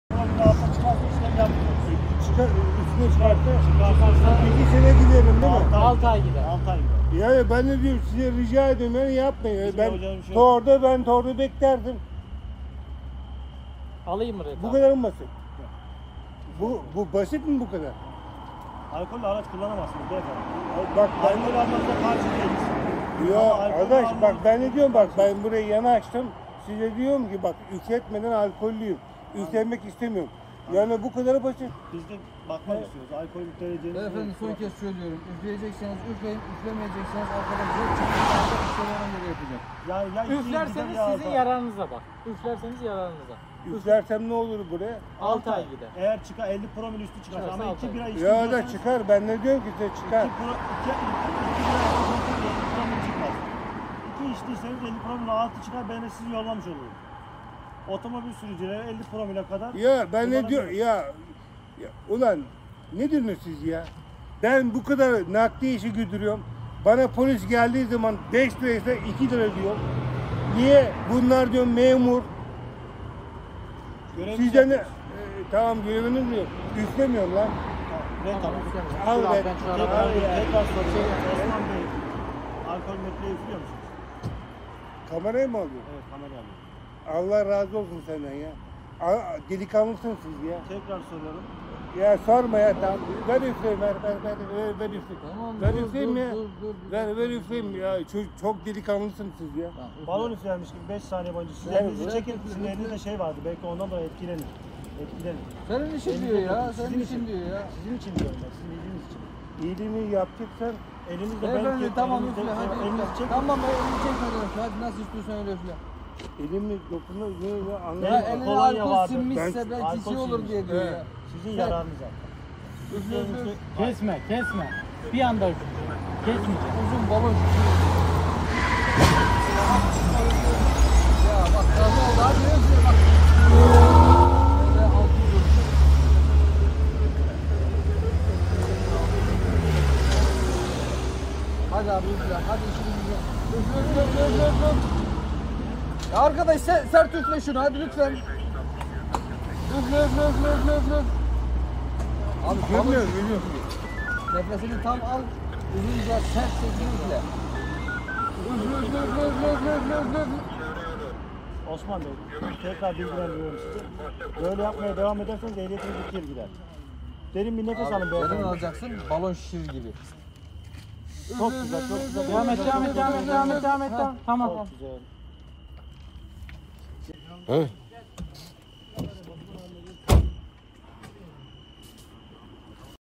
Polisin alkollü sürücü ile imtihanı
Bursa'nın İnegöl ilçesinde trafik ekiplerince durdurulan aracın sürücüsüne alkollü araç kullanmaktan bin 823 TL cezai işlem uygulandı. Alkol metreyi üflememekte ısrarcı olan sürücü, polis ekiplerine zor anlar yaşattı.